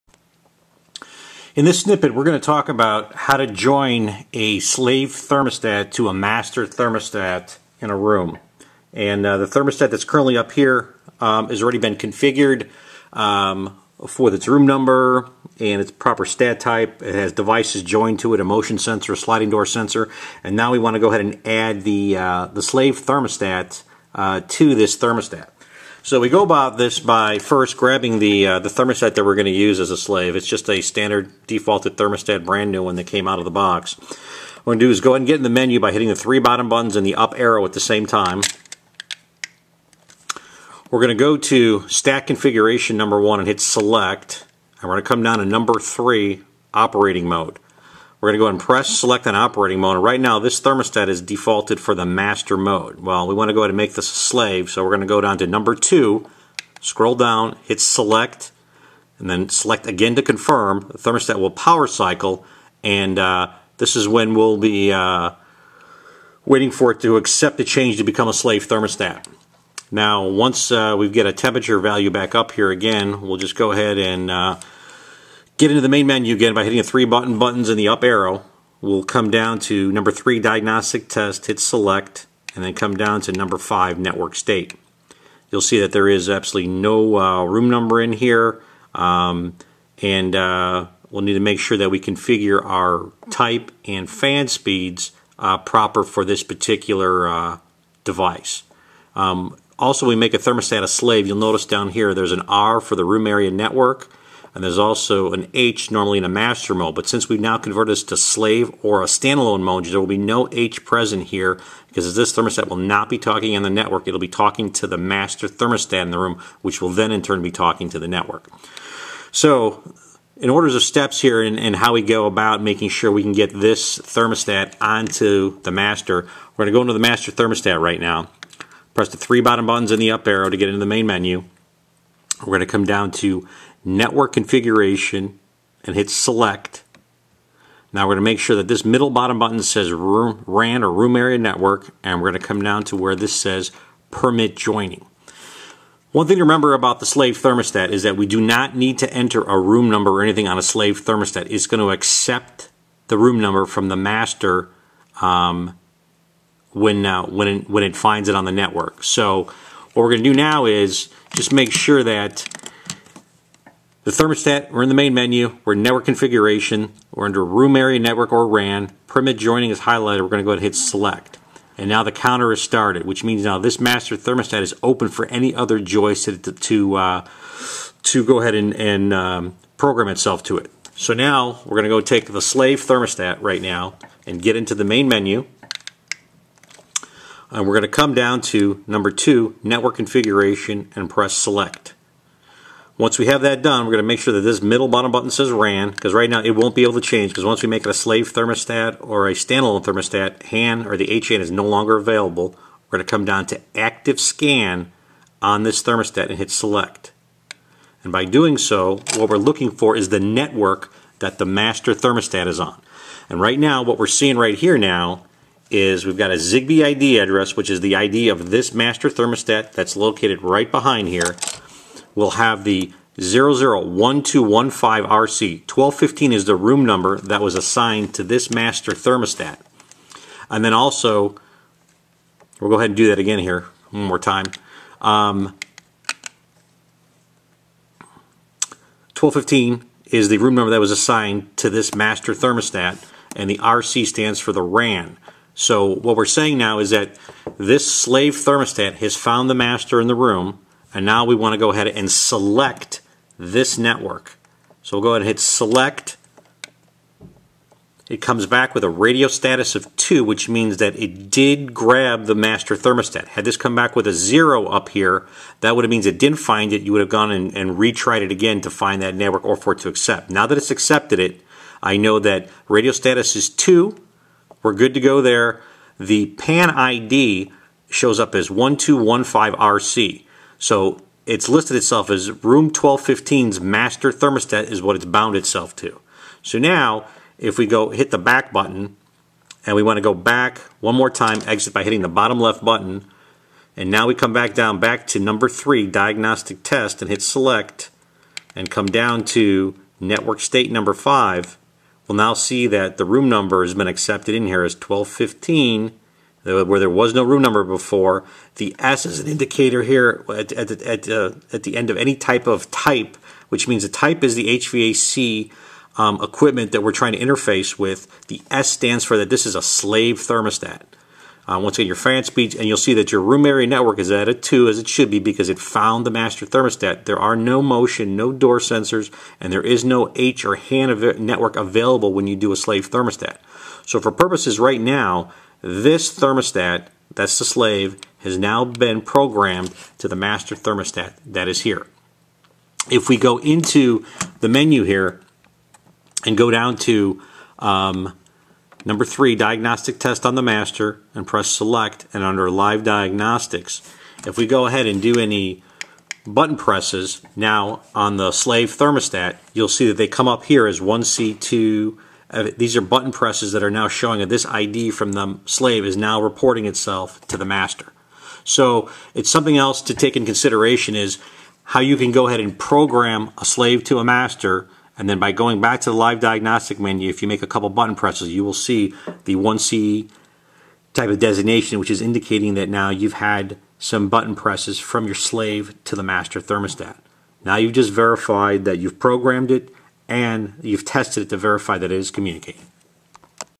Set Up #5- Master/Slave Tstat Programming
Describes how to program a slave thermostat to the master thermostat.